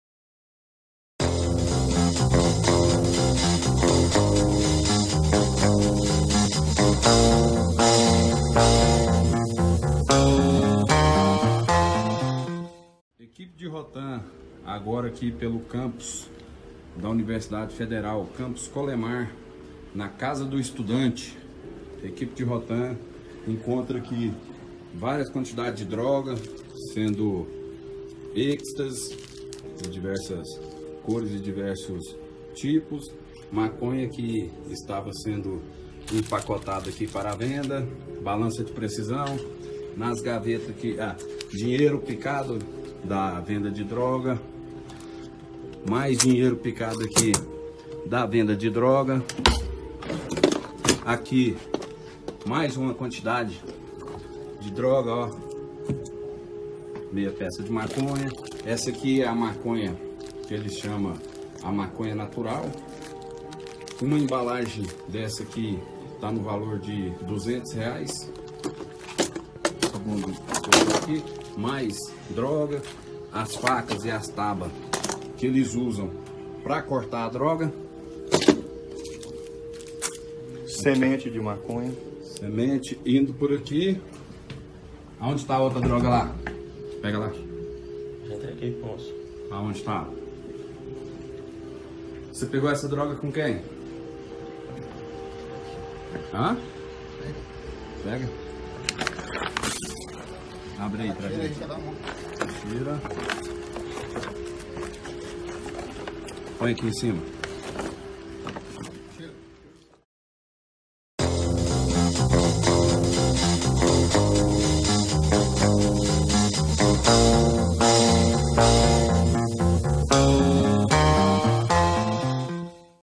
MUITA MACONHA NA CASA DO ESTUDANTE ( 21 / 05 / 2019 ).
SEGUNDO O DIÁRIO DE GOIÁS ) AGENTES DA ROTAM APREENDERAM 5 QUILOS DE MACONHA E 130 COMPRIMIDOS DE ECSTASY NO INTERIOR DA UNIVERSIDADE FEDERAL DE GOIÁS ( UFG ). PARTE DOS ENTORPECENTES FOI LOCALIZADA NA CASA DO ESTUDANTE UNIVERSITÁRIO, SITUADA NAQUELA INSTITUIÇÃO DE ENSINO SUPERIOR.